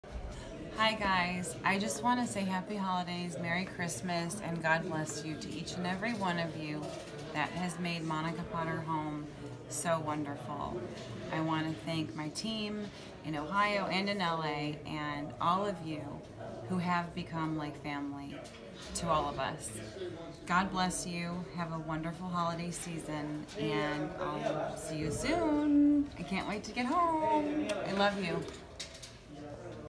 A Message From Monica: Happy Holidays and Thank You!
As 2017 comes to an end, I want to thank each and every one of you for your support, loyalty, and friendship. Whether my acting career or my company, Monica Potter Home, I couldn't do any of this without the support of all of you. We have some exciting things happening in 2018 and can't wait to share them with you! Thank you for following along on our journey. Happy Holidays, Merry Christmas and God Bless you! XO Monica